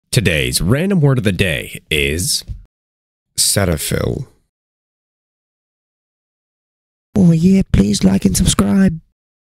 How to pronounce Cetaphil?(SORT OF CORRRECTLY...)
Welcome!
Throughout the entirety of my channel, you will learn to pronounce vocabulary the correct way. In today’s video, you will learn to pronounce Cetaphil the correct way.
Timestamps below:
0:00 intro
0:02 word of the day
0:07 THE END